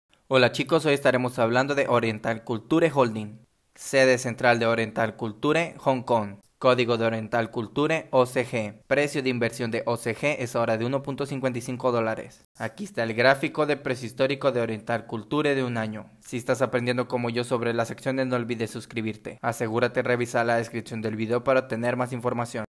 🔴 Datos Sobre El Comercio De Acciones De Oriental Culture Holdings Ltd OCG 🔴
Oriental Culture Holding LTD, a través de sus subsidiarias, opera una plataforma en línea para facilitar el comercio electrónico de obras de arte y coleccionables en China. La empresa facilita la comercialización por parte de clientes individuales e institucionales de diversos objetos de colección, obras de arte y productos básicos en sus plataformas en línea.